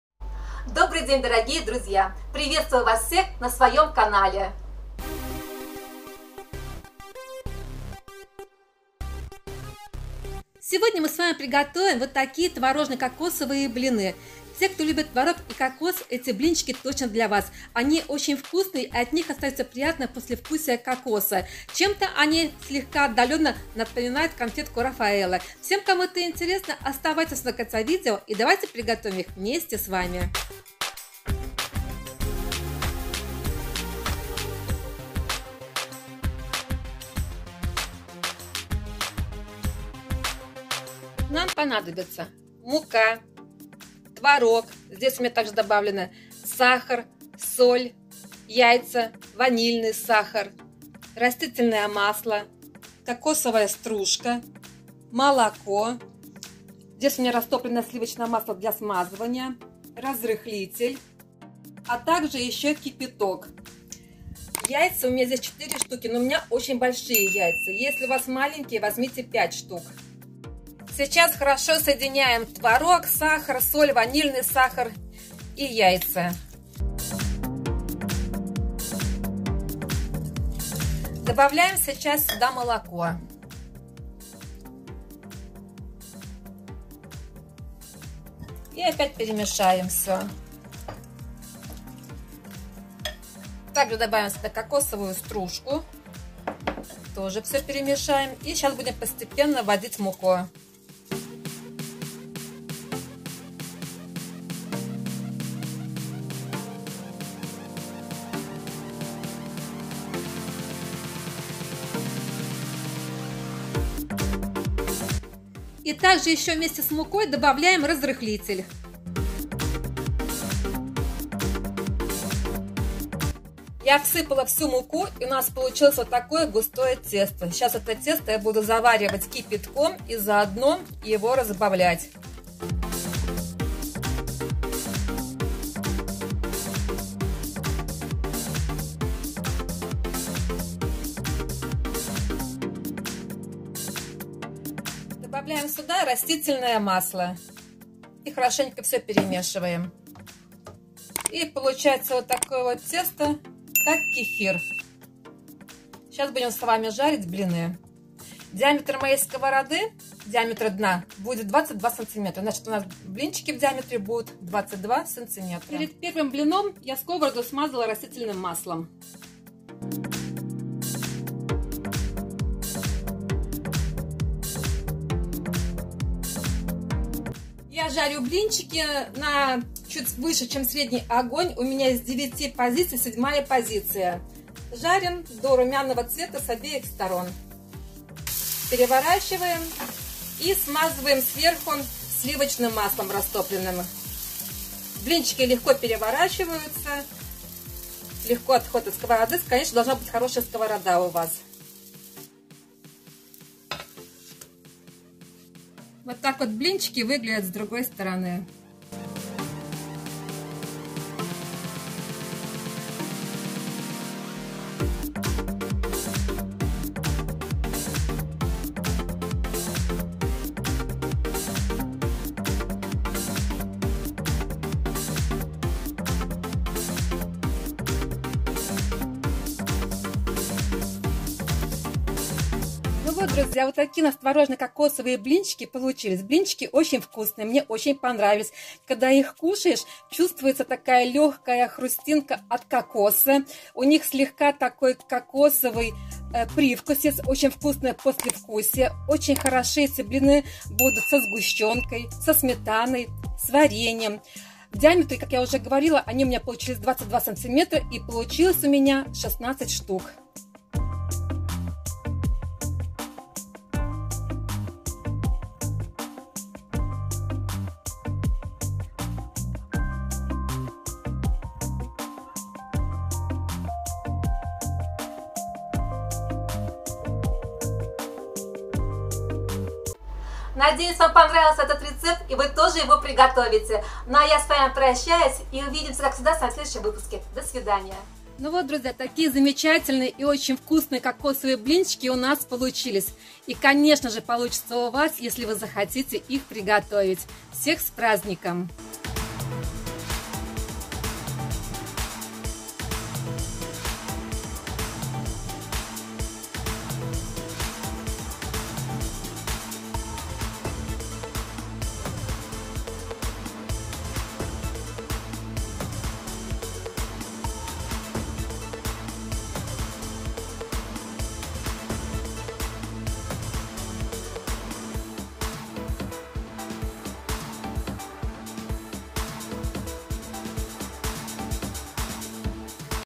Ванильные Творожно - Кокосовые Блины / Vanilla Curd - Coconut Pancakes
ИНГРЕДИЕНТЫ:
Кокосовая стружка  - 30 гр.
Масло сливочное для смазывания
Получается 16 блинов в диаметре 22 см.

INGREDIENTS:
Cottage cheese - 300 gr.
Flour - 500 gr.
Coconut flakes - 30 gr.
Eggs - 4-5 pcs.
Milk - 300 gr.
Boiling water - 600 ml.
Sugar - 4 tbsp
Vanilla sugar - 1 tsp
Vegetable oil - 50 gr.
Butter for lubrication
It turns out 16 pancakes in diameter 22 cm.

Мой хэштег в INSTAGRAM
Вы можете зарабатывать на YouTube на своих видео:
Подключайтесь к партнёрской программе :